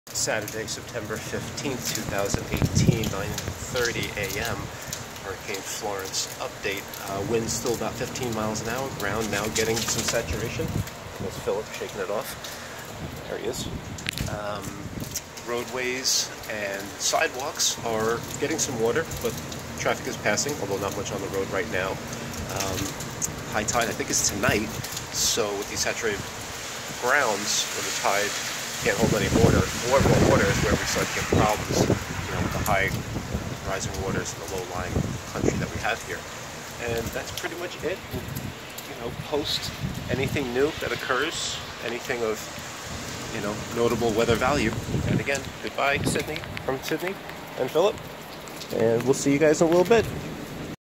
Rhodesian Ridgeback & Hurricane Florence
Third part of my updates on Hurricane (now tropical storm) Florence. Although downgraded from a Cat 2 to a Cat 1 upon landfall in N.C. yesterday, this slow moving storm continues to hover over the Low Country with continual precipitation and rain.